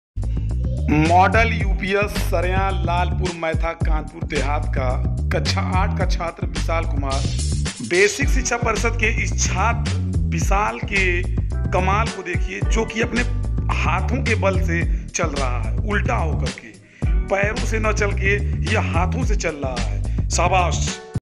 उल्टा होकर हाथों के बल पर चलता हुआ बेसिक का छात्र विशाल कुमार
मॉडल UPS सरैयां लालपुर,मैथा,कानपुर देहात का छात्र विशाल कुमार ,कक्षा 8